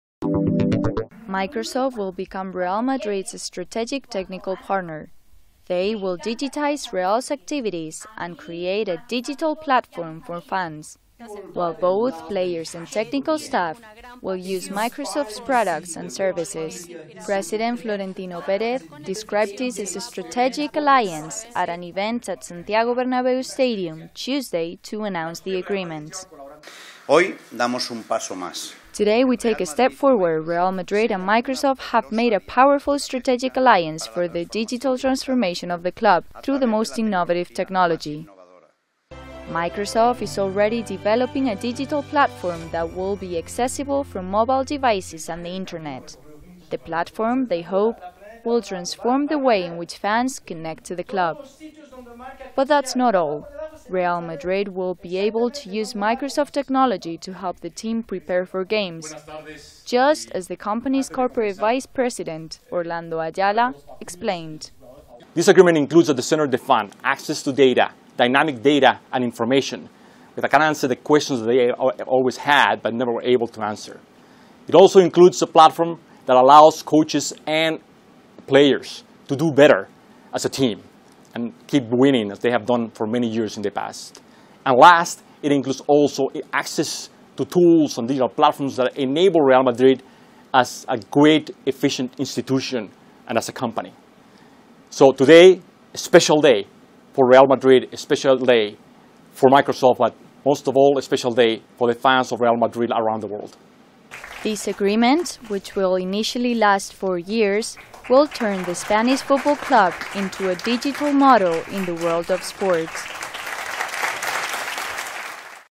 Real Madrid and Microsoft sign agreement on Spanish football club's "digital transformation"
Madrid, 18 Nov (EFE).- Microsoft announced Tuesday an agreement with Spanish football club Real Madrid to become the team's "strategic technological partner". The company plans to digitize Madrid's activities and create a digital platform for fans, while both players and technical staff will use Microsoft's products and services.